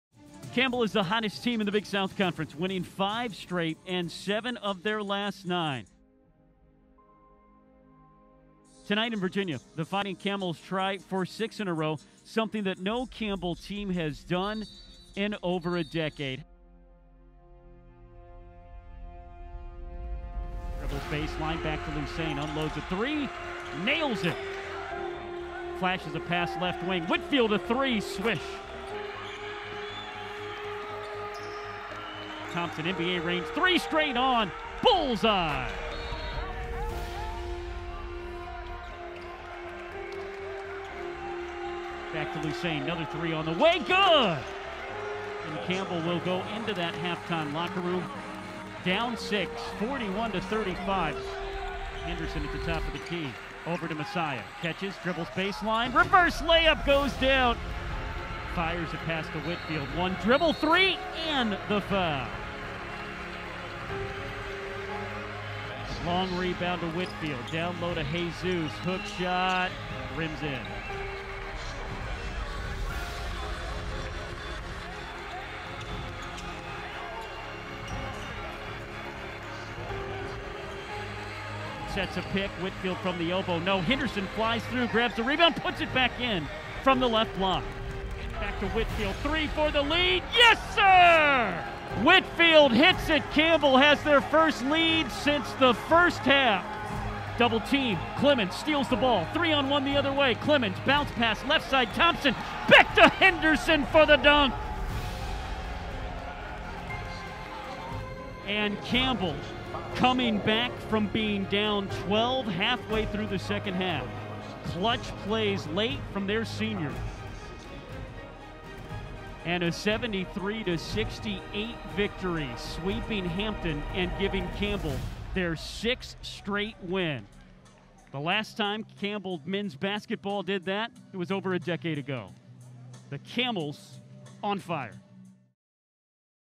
Campbell Basketball at Hampton | Cinematic Recap - 2/19/21
Campbell erases a 12 point deficit to beat Hampton 73-68.
Jordan Whitfield - 20 points
Cedric Henderson - 16 points
Joshua Lusane - 12 points
Messiah Thompson - 10 points